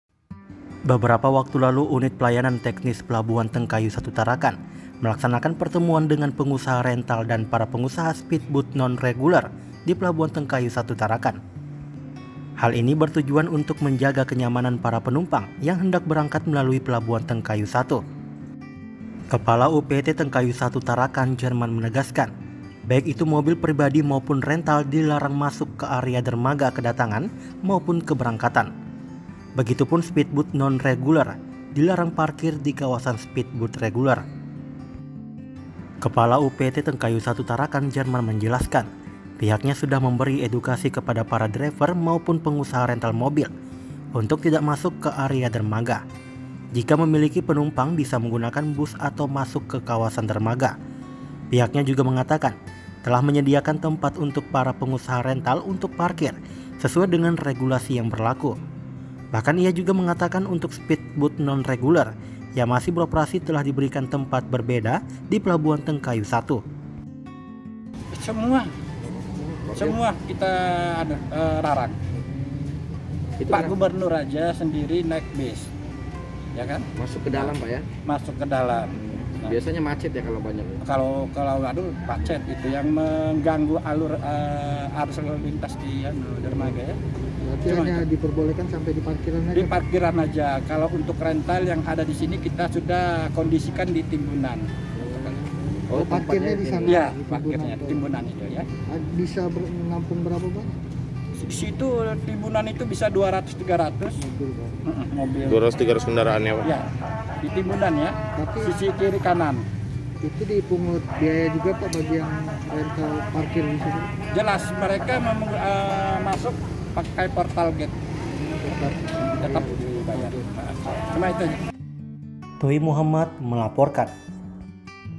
KENDARAAN PRIBADI DAN RENTAL DILARANG MASUK AREA PELABUHAN
SPEEDBOAT NON REGULER DILARANG PARKIR DI KAWASAN PELABUHAN

BEBERAPA WAKTU LALU UNIT PELAYANAN TEKNIS PELABUHAN TENGKAYU I TARAKAN MELAKSANAKAN PERTEMUAN DENGAN PENGUSAHA RENTAL DAN PARA PENGUSAHA SPEED BOAT NON REGULER DI PELABUHAN TENGKAYU 1 TARAKAN.